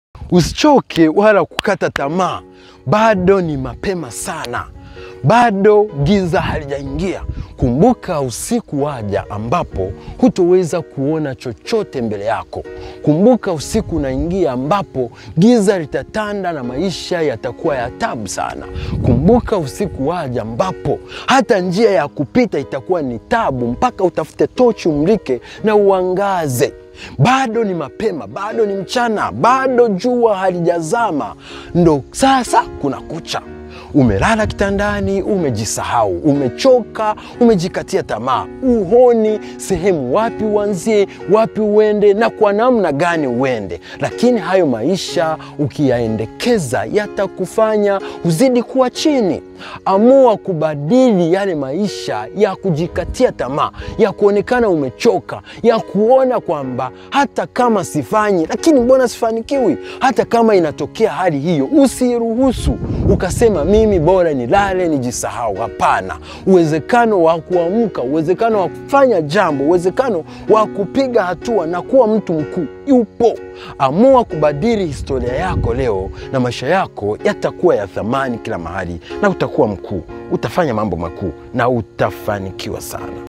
JINSI YA KUUSHINDA UCHOVU NA KUKATA TAMAA - Johaness John
jinsi Ya kuushinda uchovu na kukata tamaa

usiache kufanya

amua kuwa wa tofauti

unaweza kutoboa

usiache kufanya

tengeneza maisha yako
badi jinsi unavyojiona

unaweza kuwa juu

unaweza kuwa  Star 🌟

anza na kile kimono ulichonacho na hapo ulipo

usiache kufanya hata kama umechoma

mafanimio, matokeo, hayo yote yatawezekana ukiamua kufanya kitu leo

usijisahau

amuka toka usingizini

success is waiting for you

gont sop there you can make it.